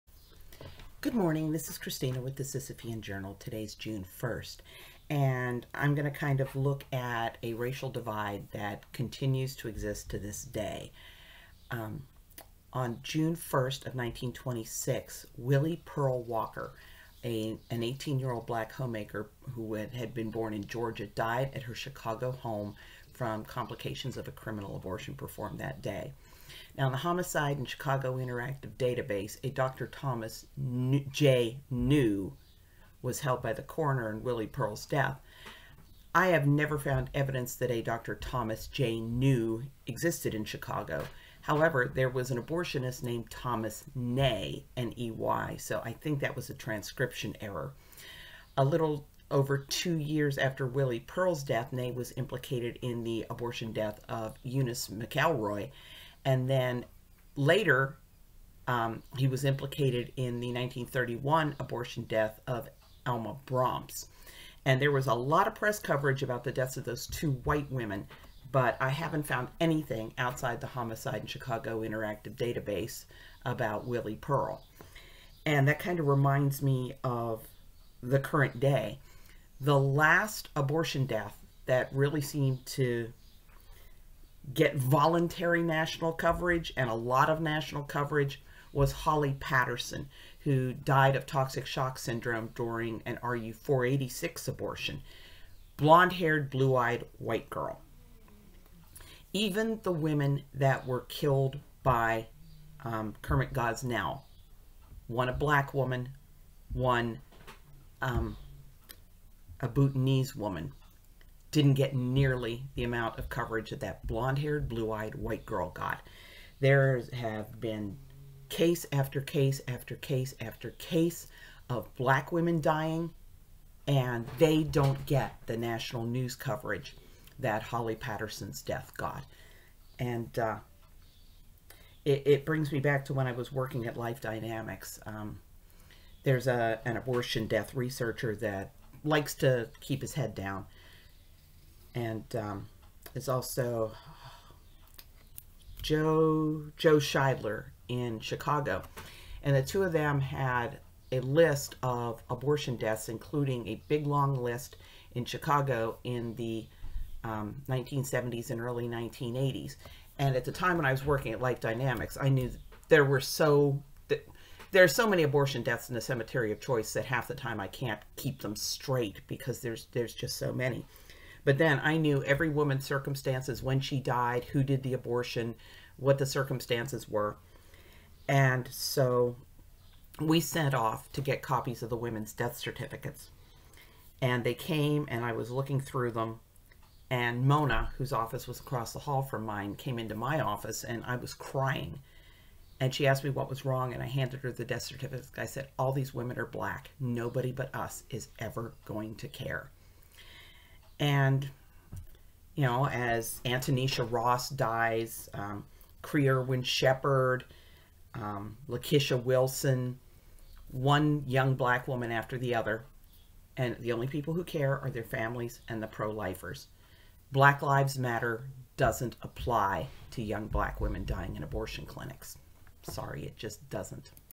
An Early Black-Lives-Don't-Seem-to-Matter Moment
When Willie Peal, an 18-year-old Black woman, died in Chicago in 1926, there was zero news coverage. Her abortionist was most likely Dr. Thomas J. Ney. A little over two years after Willie Pearl's death, Ney was implicated in the death of a white woman and the news covered the case. Ditto the case of another young white woman a few years later. This reminds me of how the national press covered the abortion death of blonde-haired blue-eyed Holly Patterson, which the deaths of young Black women like Antonesha Ross, Tonya Reaves, TaTanisha Wesson, and Cree Erwin-Sheppherd are local stories of they're covered at all. Not even the two women who died under Kermit Gosnell's dubious care -- one Black, one Bhutanese -- got anywhere near the press coverage generated by the young white woman's death.